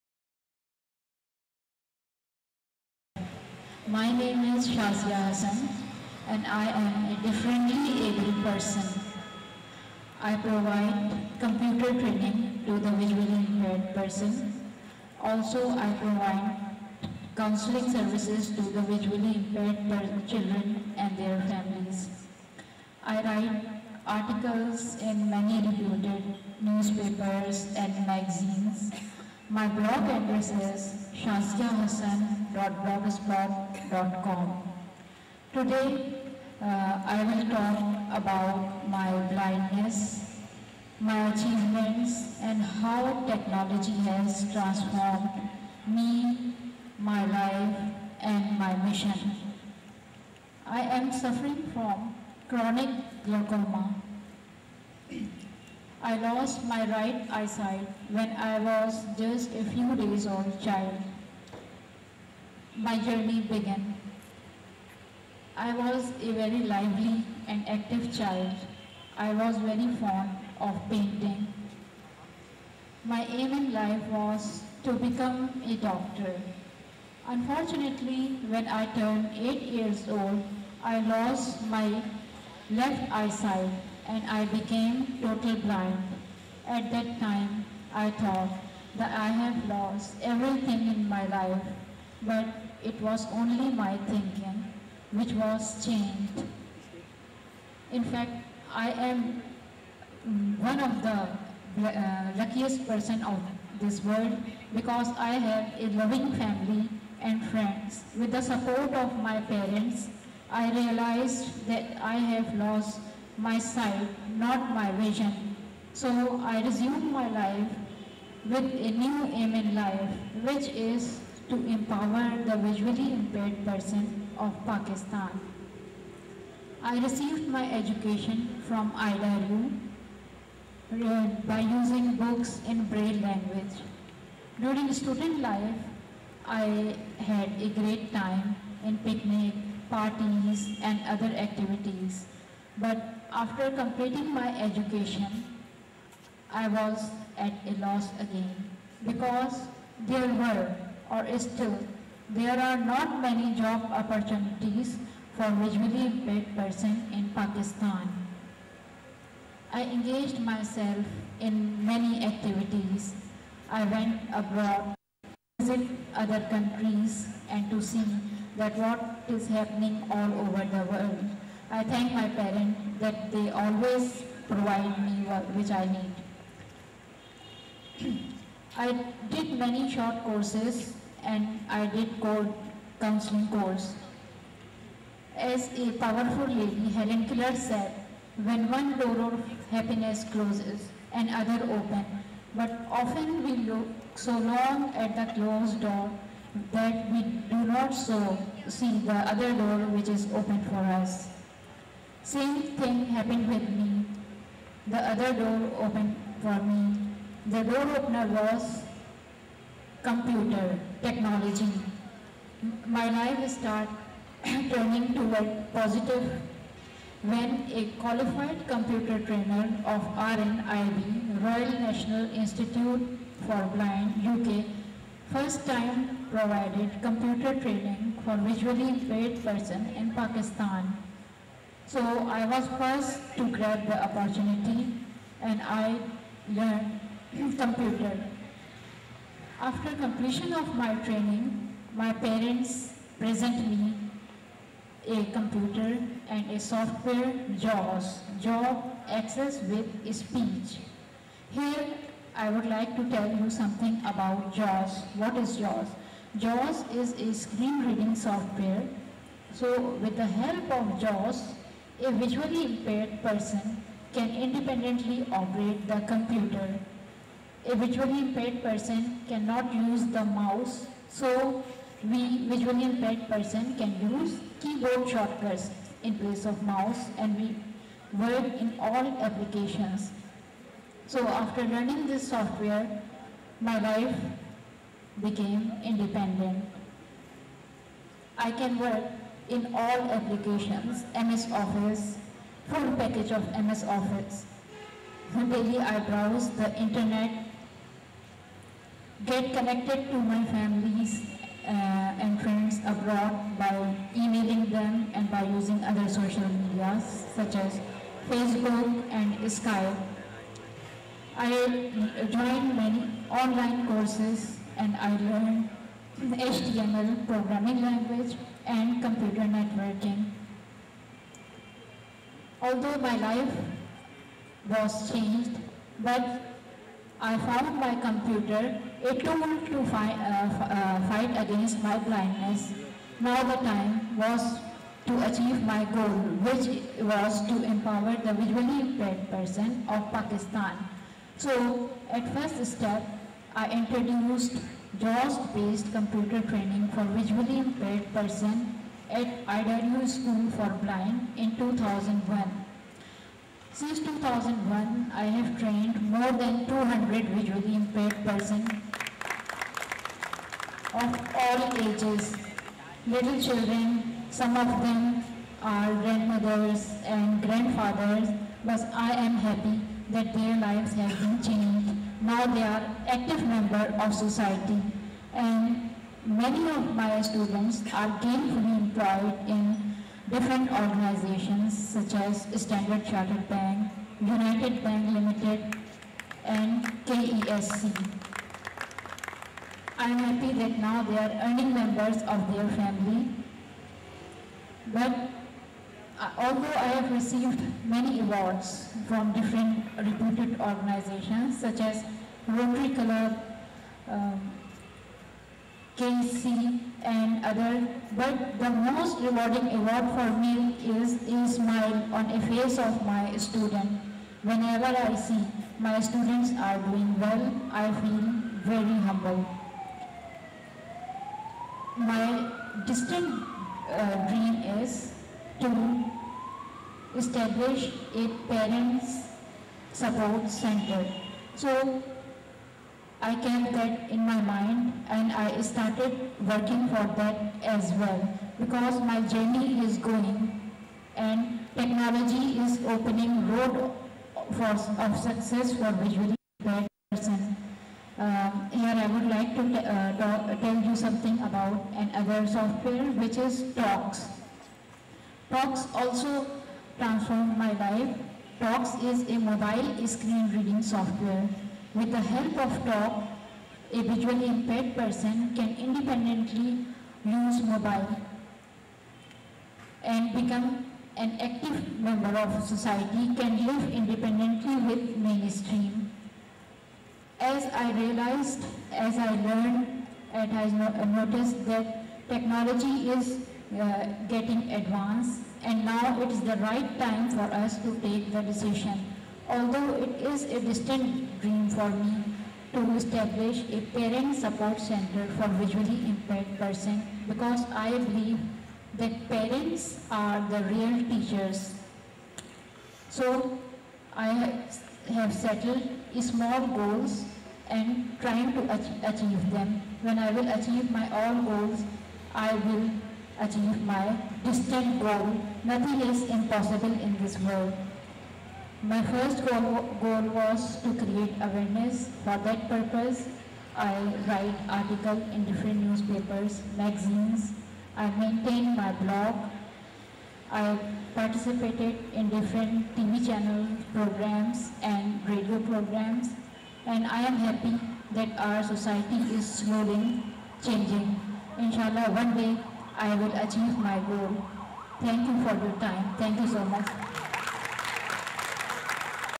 TEDxKorangi - Shazia Hassan
Shazia Hassan is the first blind computer user and trainer in Pakistan. She has trained more than 200 blind persons who can independently use computer for internet, email, browsing etc. Many of the Blind Students trained with Computer Skill are employed leading institutions like Standard Chartered Bank,United Bank Ltd, Karachi Electric Supply Company.